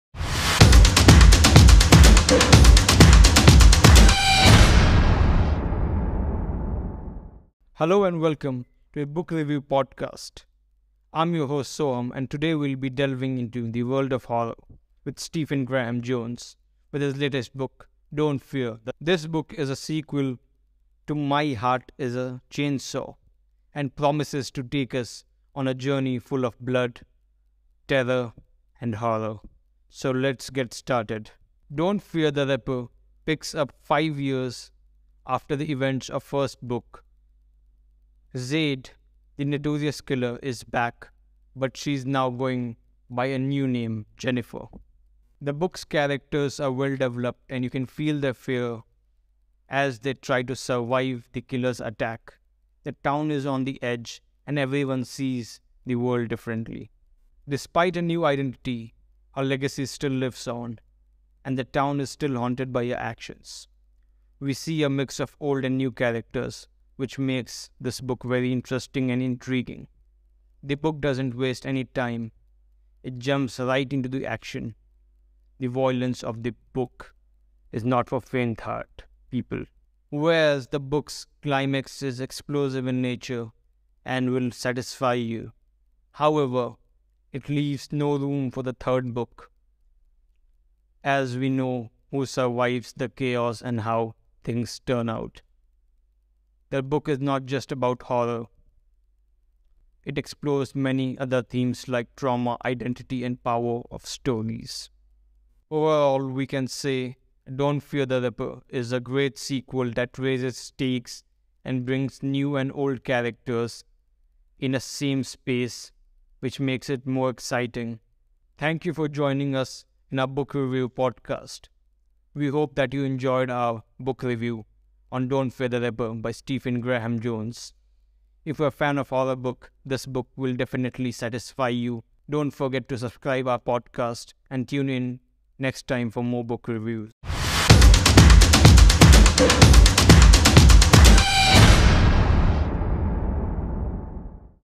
Don't Fear the Reaper by Stephen Graham Jones | Booklicious Podcast | Episode 32
Don't Fear the Reaper by Stephen Graham Jones | Booklicious Podcast | Episode 32 | Powered By gobookmart.com

Hello and welcome to the Book Review podcast. I'm your host and today we're diving into the world of horror with Stephen Graham Jones' latest book, "Don't Fear the Reaper". This book is a sequel to "My Heart Is a Chainsaw" and promises to take us on a journey full of blood, gore, and terror. So, let's get started!